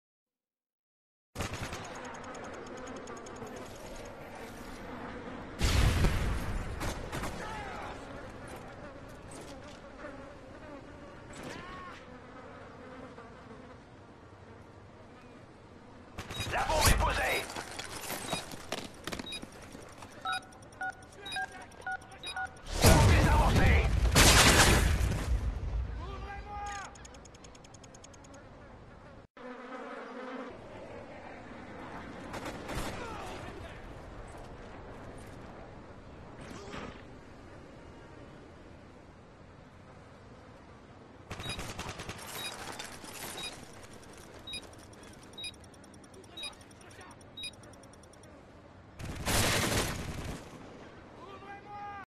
♠ Ninja Defuse ! ♠
Une petite Ninja defuse a vous mettre sous la dent pour attendre la vidéos spéciale 100  Abonnés ! Donc n'oubliez pas le j'aime a la fin qui fait plaisir Voila ! Et faite tourné la vidéos a vos amis !  Ne mettez pas de pouce rouge svp juste expliquer moi se qui ne vous plait pas !